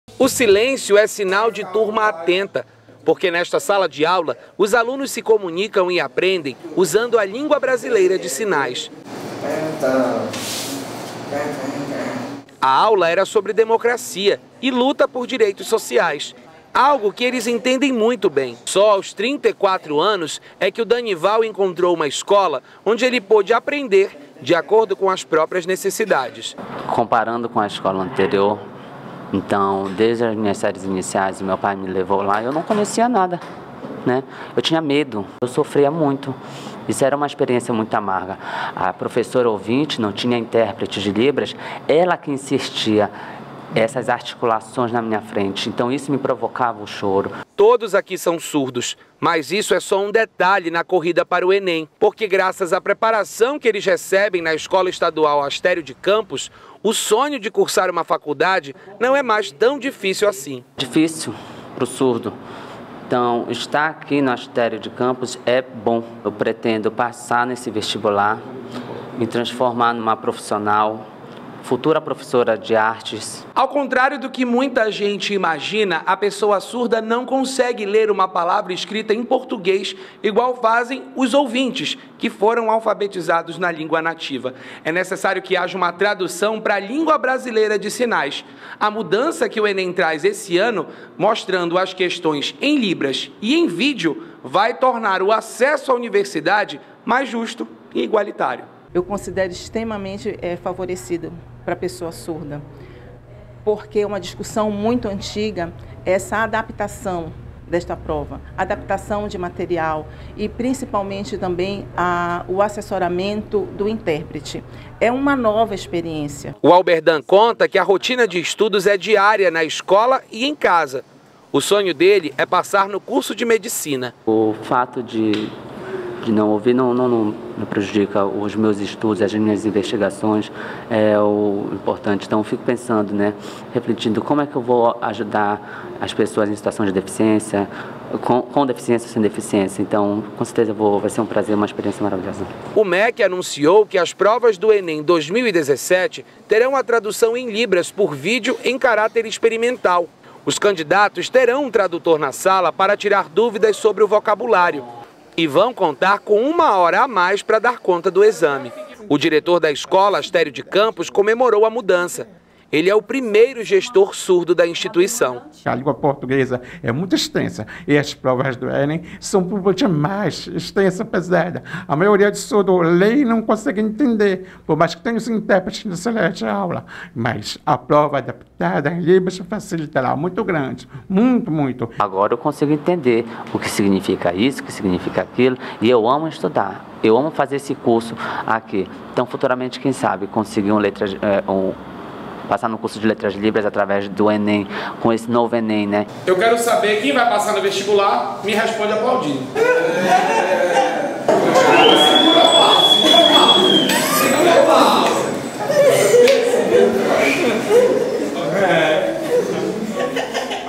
SBT PARÁ (12.04.2017) ENEM: alunos surdos terão provas em libras e vídeo
Alunos surdos comemoram uma mudança no edital do Exame Nacional do Ensino Médio. A partir deste ano, as questões  vão ser traduzidas em libras e apresentadas em vídeo. A novidade animou os estudantes que sonham em ingressar em uma universidade..